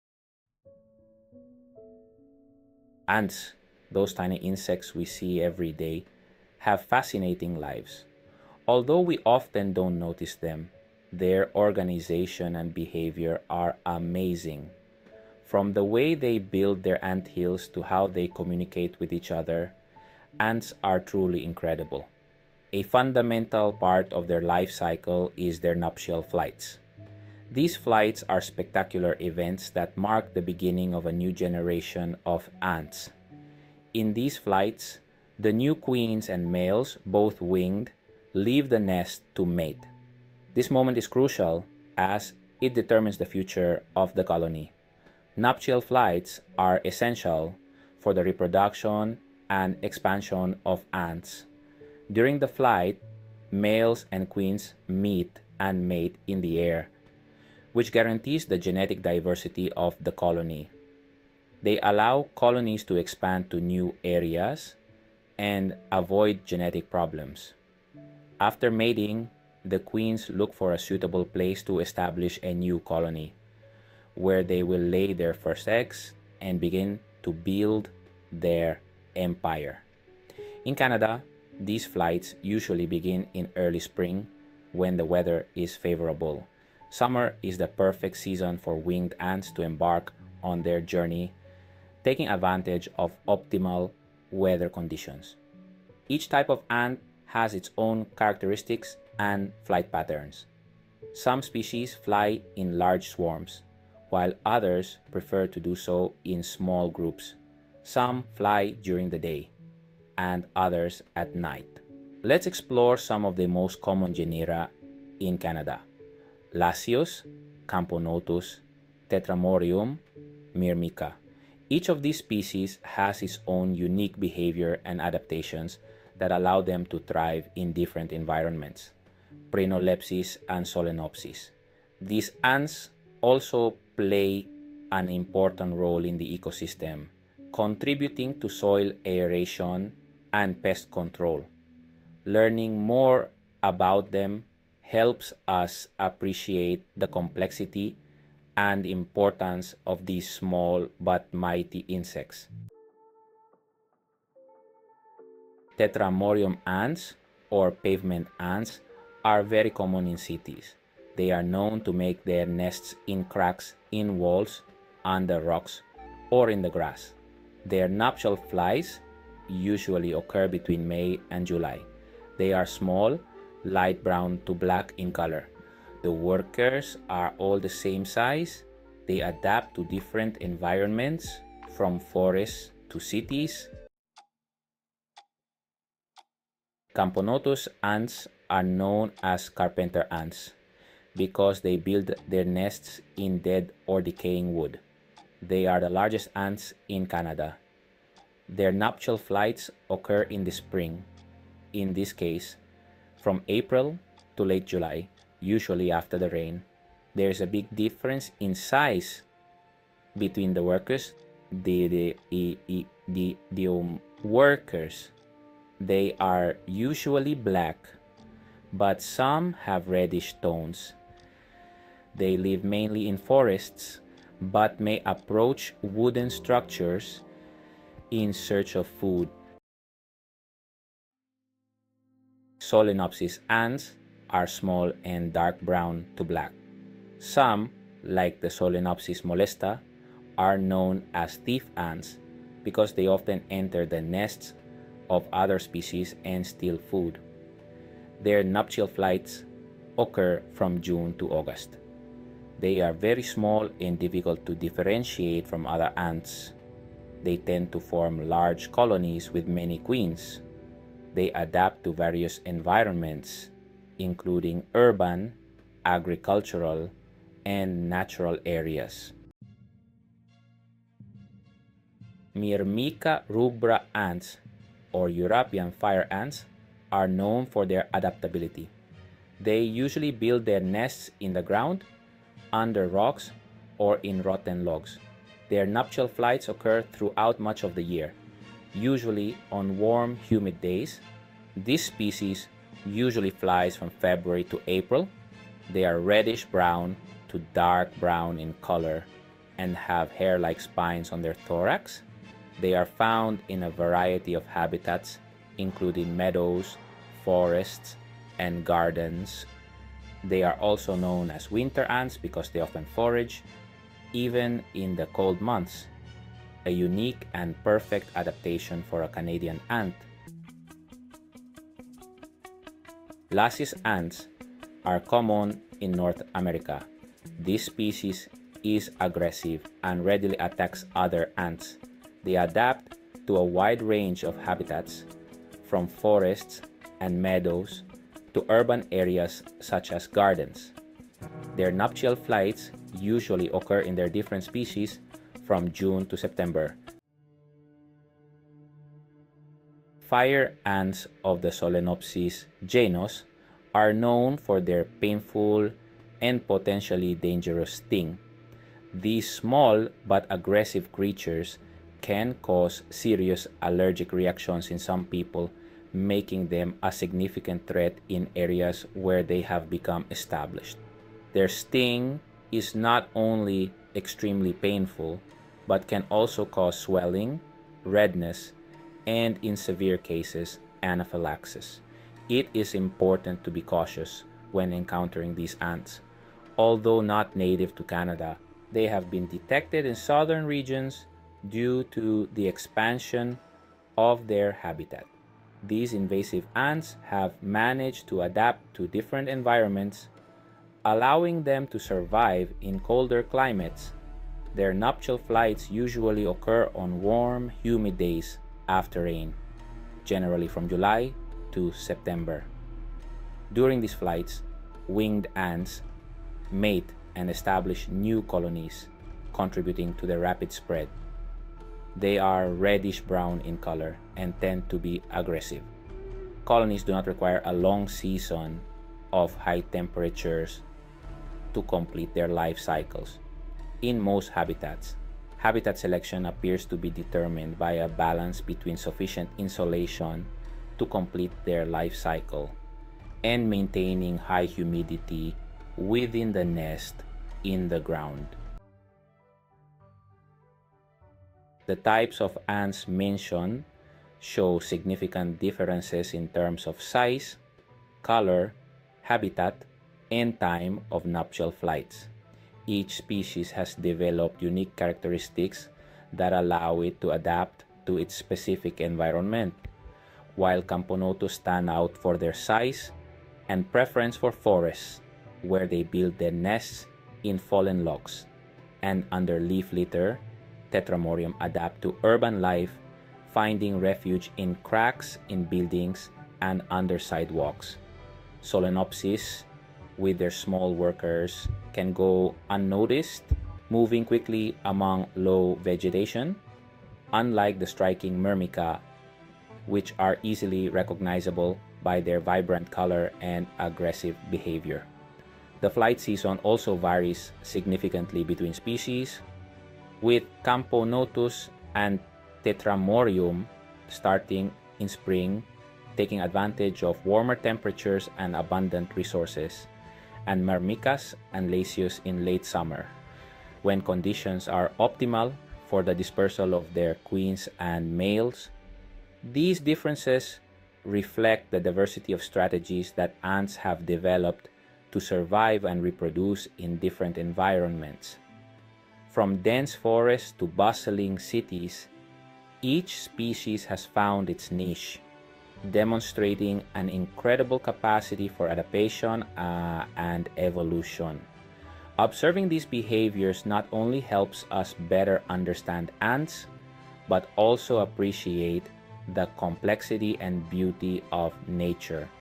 CANADA'S NUPTIAL FLIGHT SEASONS
In this video I will be sharing information about what nuptial flights are, in which we will find our possible queen ants, I will give basic information about the seasons where they have their greatest peak as a mating season.